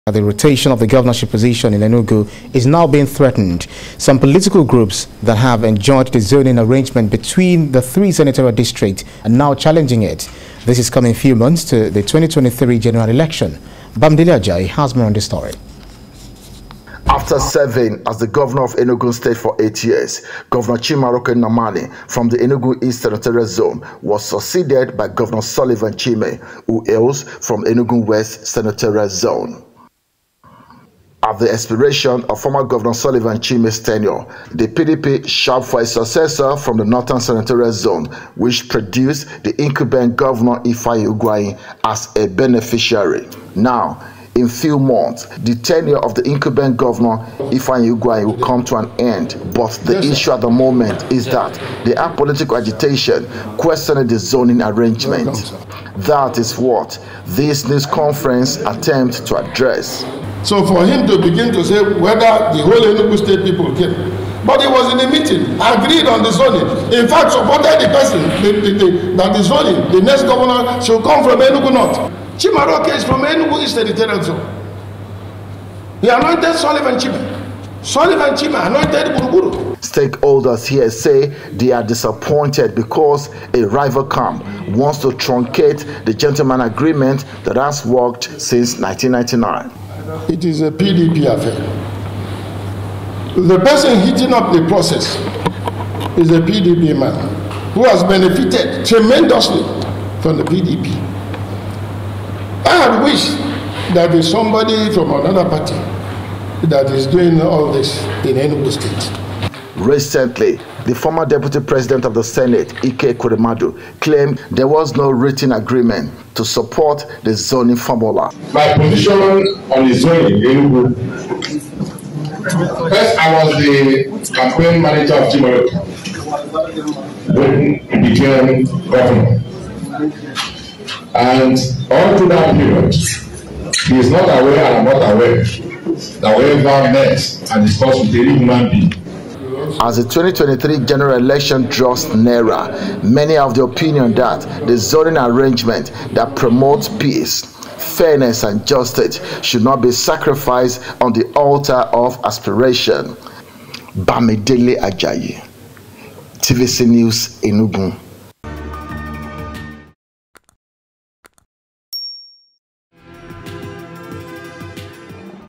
No Written Agreement on Enugu PDP Governorship Zoning - Ike Ekweremadu
The rotation of the Governorship position in Enugu is now been threatened as some political groups that have enjoyed the zoning arrangement between the three senatorial districts are now challenging it.